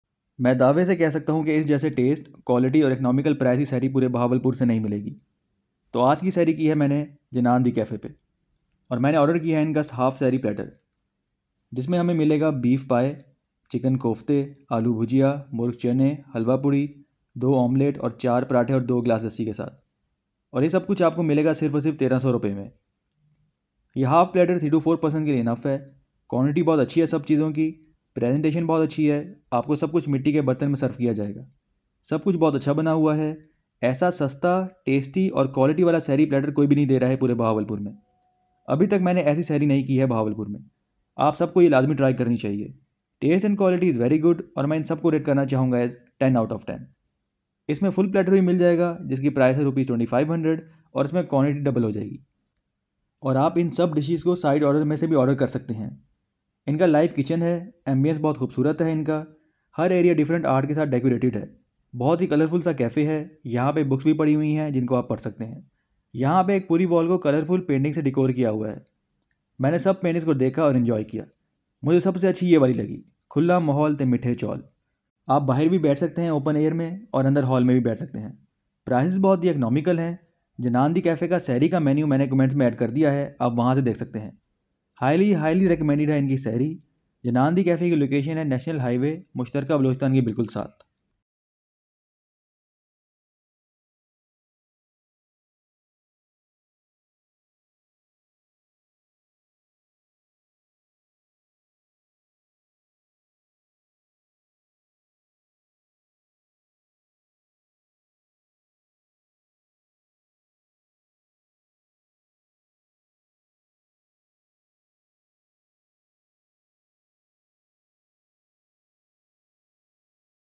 Janan De Cafe | Sehri Platter | Bahawalpur | Lodhran
Janan De Cafe. ❤️

In This Video You Will See Their Sehri Platter And Ambiance Review. 😍

I Ordered Their Half Platter

Beef Paaye… Chicken Kofte… Alu Bhujiya… Murgh Channey… Halwa Puri… 2 Omelette… 4 Parathay And 2 Glass Lassi.

Only In Rs 1300

Taste And Quality Was Very Good.  10/10

Prices Are Very Economical

Ambiance Is Beautiful And Colourfull.

Service Is Excellent.

Menu Is Added In The Comments.

Location: National Highway… Beside Mushtarka Balochistan Restaurant.